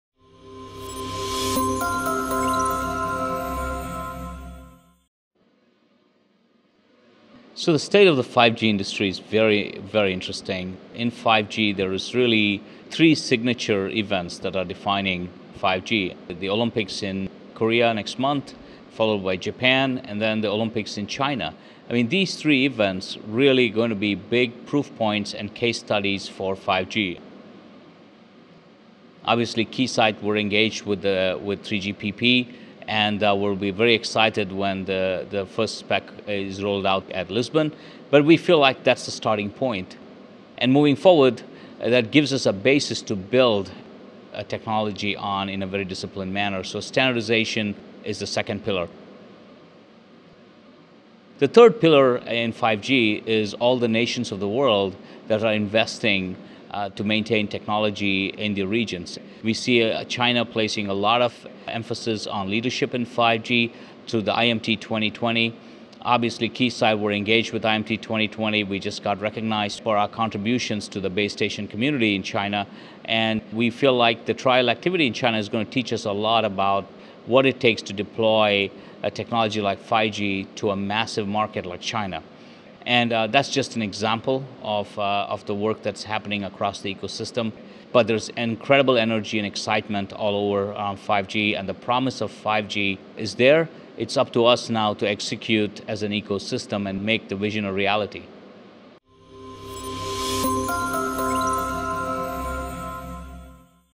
Keysight senior VP Satish Dhanasekaran: the state of the 5G industry
Keysight senior vice-president Satish Dhanasekaran, at the Keysight 5G Tech Connect event in December 2017, discusses three pillars that define the state of the 5G industry: the Olympics as signature events, standardization (5G NR), and global investments.